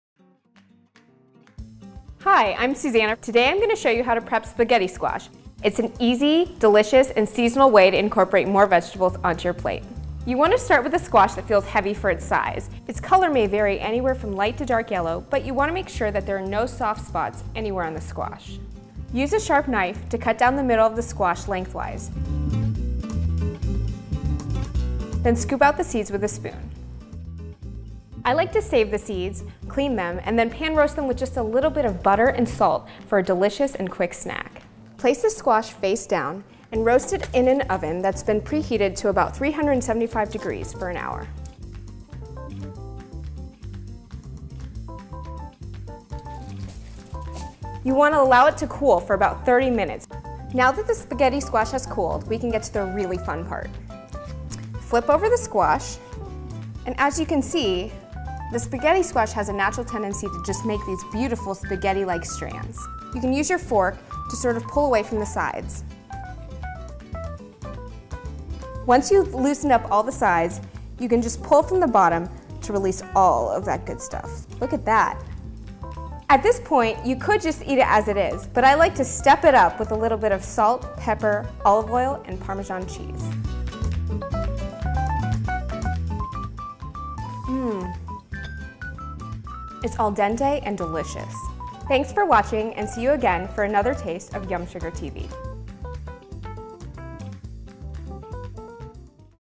How to Prepare Spaghetti Squash
Fall's the time to take advantage of spaghetti squash, which you'll find at peak season right now. Never tried cooking with spaghetti squash? Don't even be a little afraid: cooking with one is more intuitive than you'd think. And with a texture reminiscent of pasta, this vegetable is the perfect vehicle for a light olive oil and cheese dressing or any traditional pasta sauce. Watch the video to enjoy a "spaghetti" dish that's nutritious, wholesome, and gluten-free.